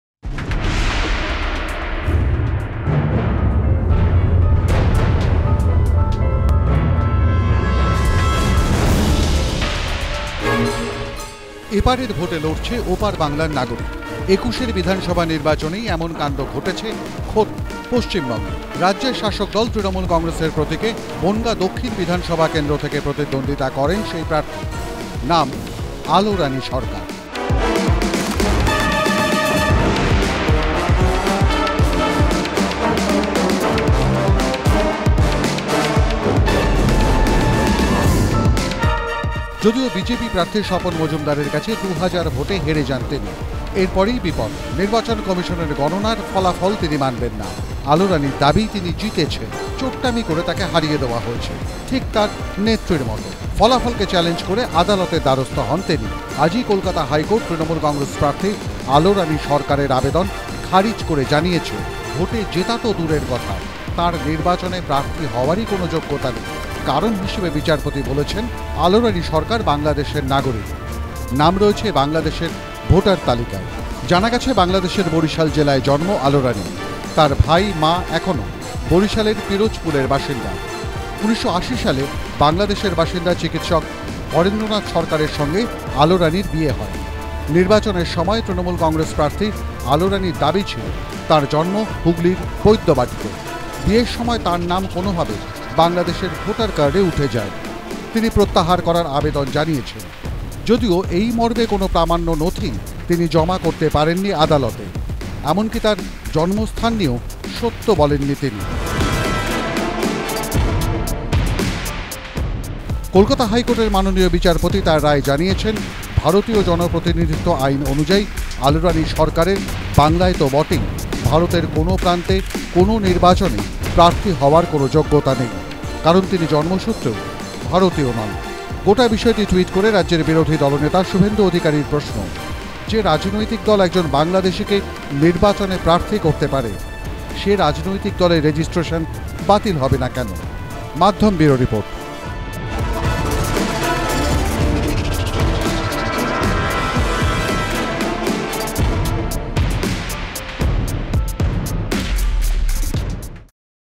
TMC Bangladeshi Candidate: ভোটে তৃণমূলের 'বাংলাদেশি' প্রার্থী! হাইকোর্টে 'মুখ পুড়ল' আলো রানীর
এপারের ভোটে লড়ছে ওপার বাংলার নাগরিক! একুশের বিধানসভা নির্বাচনেই এমন কাণ্ড ঘটেছে খোদ এরাজ্যে। রাজ্যের শাসক দল তৃণমূল কংগ্রেসের প্রতীকে বনগাঁ দক্ষিণ বিধানসভা কেন্দ্র থেকে প্রতিদ্বন্দ্বিতা করেন সেই প্রার্থী। নাম আলো রানী সরকার।
যদিও বিজেপি প্রার্থী স্বপন মজুমদারের কাছে দু'হাজার ভোটে হেরে যান, এই আলো রানী সরকার।
এরপরই বিপদ! নির্বাচন কমিশনের গণনার ফলাফল তিনি মানবেন না। আলো রানীর দাবি, তিনি জিতেছেন। চোট্টামি করে তাঁকে হারিয়ে দেওয়া হয়েছে। ঠিক তাঁর নেত্রীর মতো। ফলাফলকে চ্যালেঞ্জ করে আদালতের দ্বারস্থ হন তিনি।
আজই কলকাতা হাইকোর্ট তৃণমূল প্রার্থী আলো রানী সরকারের আবেদন খারিজ করে জানিয়েছে, ভোটে জেতা তো দূরের কথা, তাঁর নির্বাচনে প্রার্থী হওয়ারই কোন যোগ্যতা নেই। কারণ হিসেবে বিচারপতি জানিয়েছেন,
"আলোরানী সরকার বাংলাদেশের নাগরিক। নাম রয়েছে বাংলাদেশের ভোটার তালিকায়।"
জানা গেছে বাংলাদেশের বরিশাল জেলায় জন্ম আলো রানীর। তাঁর ভাই-মা এখনও বরিশালের পিরোজপুরের বাসিন্দা। ১৯৮০ সালে বাংলাদেশের বাসিন্দা চিকিৎসক হরেন্দ্রনাথ সরকারের সঙ্গে আলোরানীর বিয়ে হয়। নির্বাচনের সময় তৃণমূল প্রার্থী আলো রানীর দাবি ছিল, তাঁর জন্ম হুগলির বৈদ্যবাটিতে, বিয়ের পর তাঁর নাম কোনও ভাবে বাংলাদেশের ভোটার কার্ডে উঠে যায়। তিনি প্রত্যাহার করার আবেদন জানিয়েছেন। যদিও এই মর্মে কোনও প্রামাণ্য নথিই তিনি জমা করতে পারেননি আদালতে। এমনকি তাঁর জন্মস্থান নিয়েও সত্য বলেননি তিনি।
কলকাতা হাইকোর্টের মাননীয় বিচারপতি তাঁর রায়ে জানিয়েছেন,ভারতীয় জনপ্রতিনিধিত্ব আইন অনুযায়ী, আলো রানী সরকারের বাংলায় তো বটেই, ভারতের কোনও প্রান্তে কোনও নির্বাচনেই প্রার্থী হওয়ার কোনও যোগ্যতা নেই। তিনি জন্মসূত্রেও ভারতীয় নয়।
গোটা বিষয়টি ট্যুইট করে রাজ্যের বিরোধী দলনেতা শুভেন্দু অধিকারী প্রশ্ন তুলেছেন, যে রাজনৈতিক দল একজন বাংলাদেশিকে নির্বাচনে প্রার্থী করতে পারে, সে রাজনৈতিক দলের রেজিস্ট্রেশন বাতিল হবে না কেন?
সুপারঃ বাংলা 'বাংলাদেশে'র মেয়েকেও চায়?